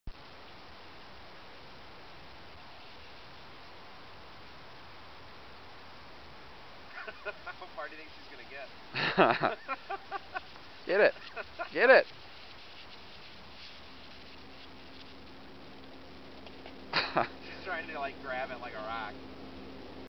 Emma Jane eating giant snowcone.AVI
Emma Jane eats a giant chunk of snow at the waterfall on Wolf Creek Pass.